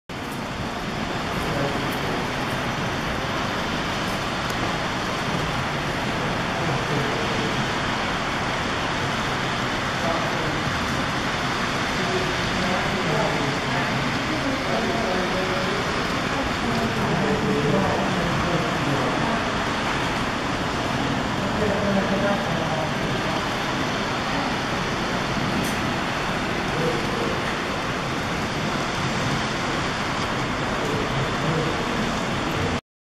N Guage Trolleys Philadelphia
2 PCC streetcars and a Brill trolley running in the lobby of SEPTA headquarters in Philadelphia